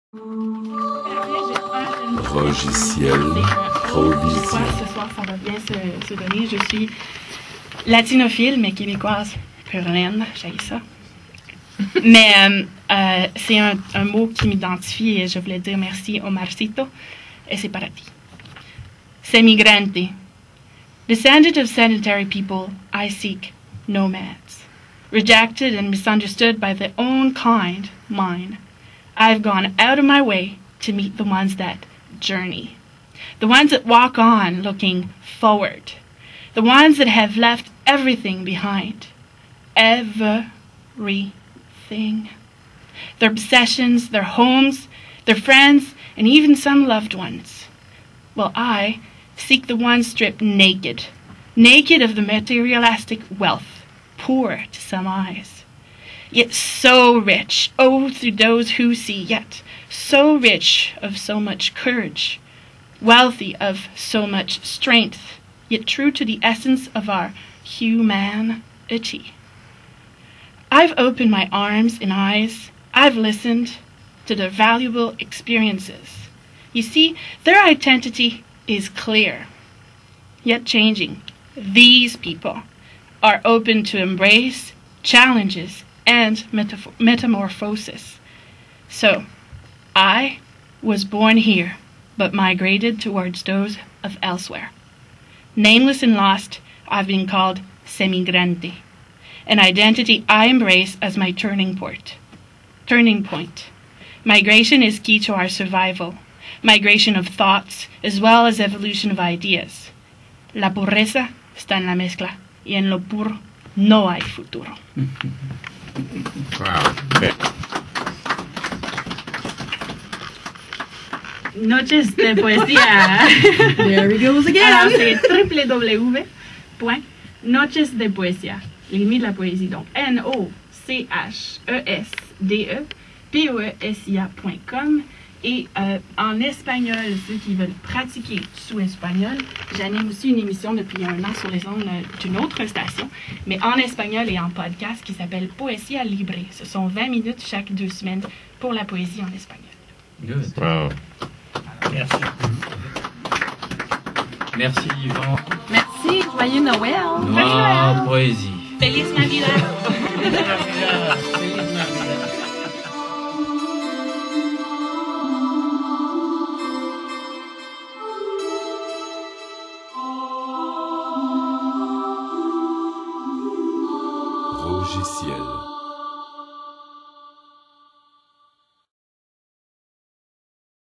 Elizabeth Robert poésie sur CHOQ FM - max 301
I am a literature addict, a recovering book worm and a beginner poet. For a few years now, I've worked at erecting bridges between cultures showcasing others' words for a while and translating them from a language to another. Now, some of my own words are asking to be heard and I've decided to let a few of them out.

Born and raised in Beloeil, Québec, Élizabeth grew up in a bilingual environment and quickly fell into the well of multiculture and languages, by choice. When other kids watched cartoons, she watched some TELE-ETHNIE. Her thing was to learn new words, new languages and new customs that were foreign to her friends. Today, Montréal is a world she embraces and feeds upon discovering.

Although she's been doing events for a while and her voice has been heard reading other people's words. (Éliz is a literary translator, an MC and a host first and foremost...) Lately, with the approach of her year 30, she has ventured into unleashing words of her own. She writes in French, English and Spanish.

Inspired by contemporary Canadian poets and performers, Éliz hopes to have fun along the path called life. Blessed by the friendships of artists and kindhearted spirits, she aims to do just that.